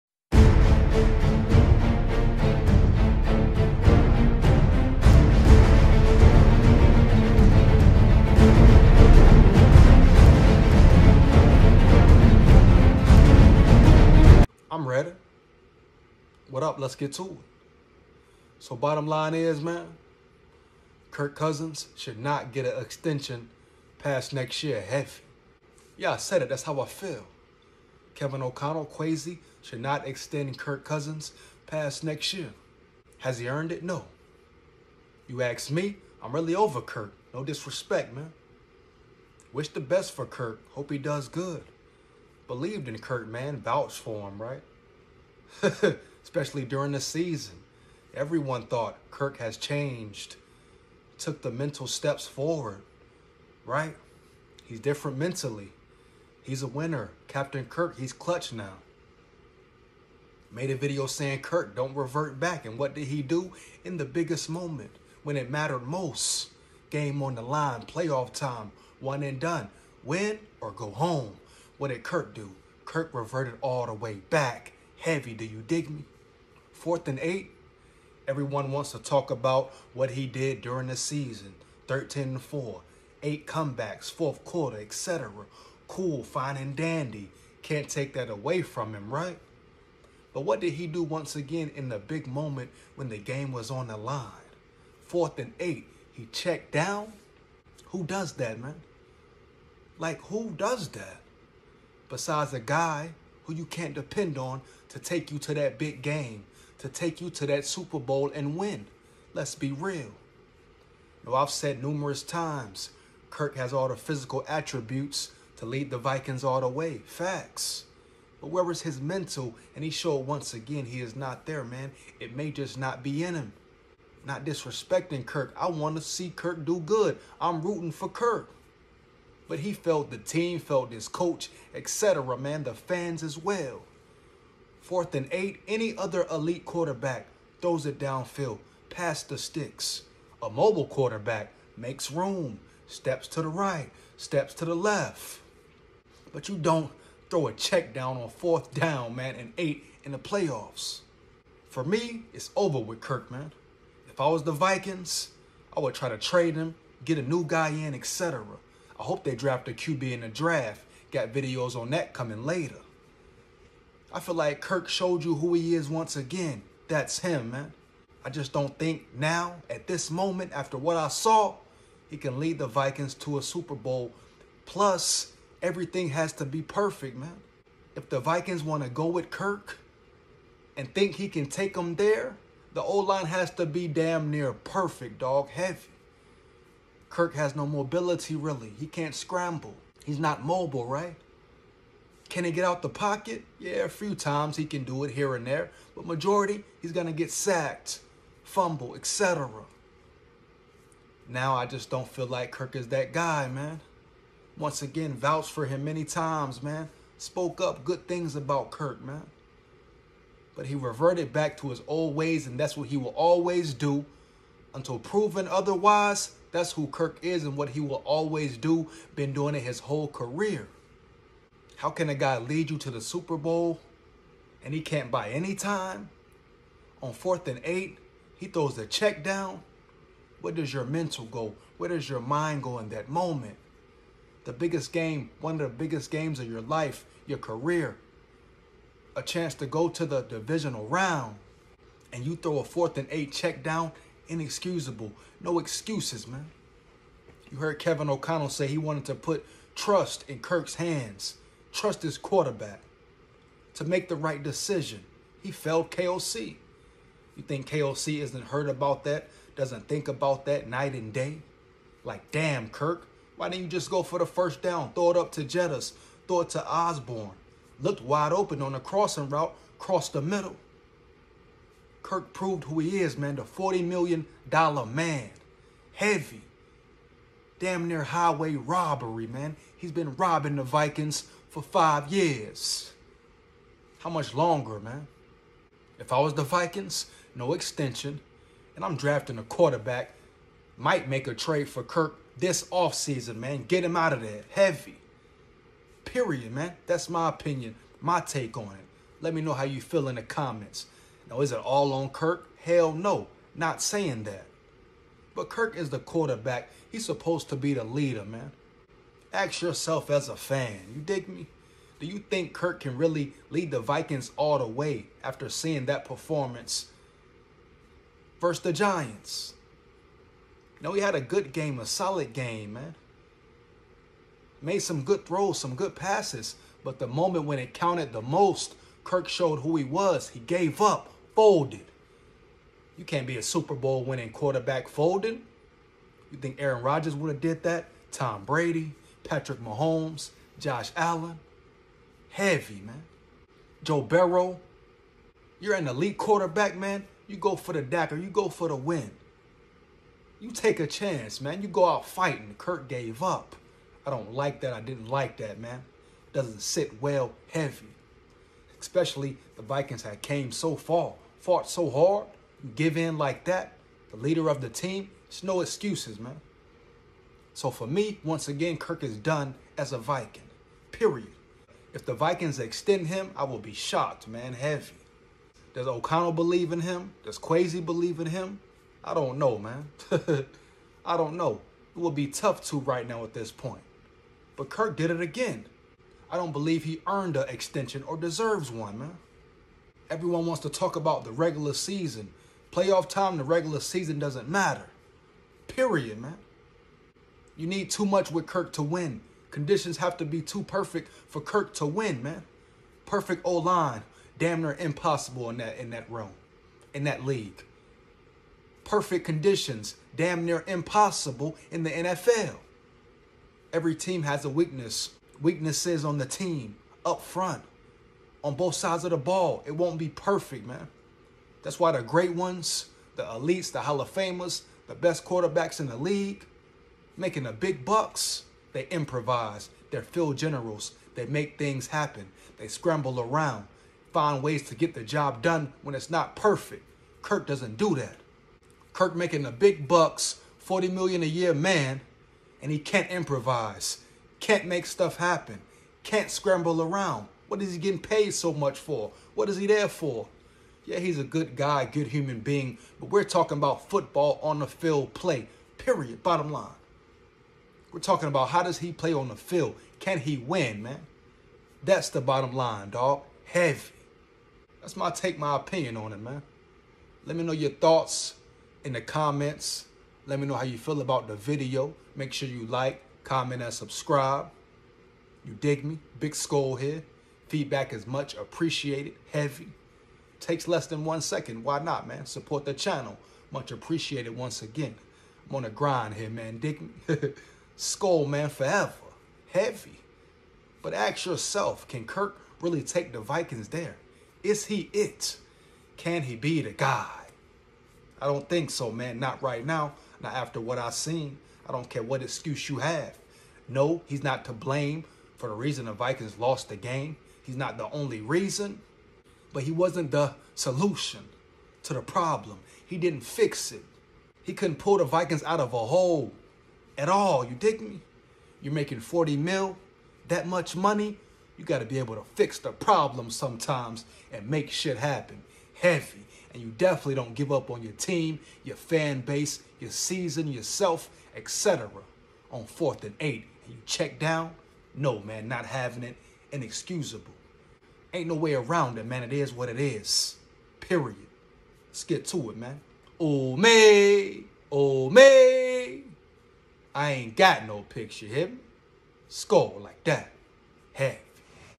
Minnesota Viking Kirk Cousins should NOT get an extension..Matter of FACT he should GO.HEAVY.SKOL💯
The Minnesota Vikings..
Should NOT..
Give Kirk Cousins..
And extension..
Matter of FACT..
They should figure..
Out a way..
To get him out of there..
He quit on his team..
His fans..
The organization..
And himself..
He showed who he is..
HE IS WHO HE IS..
Enough is enough..
He proved..
Once again..
That he cannot take..
The Vikings all the way..
He reverted back..
Big time..
No excuses..
Time for the Minnesota Vikings..
To go..
A different ROUTE..
HEAVY..
SKOL💯💪🏾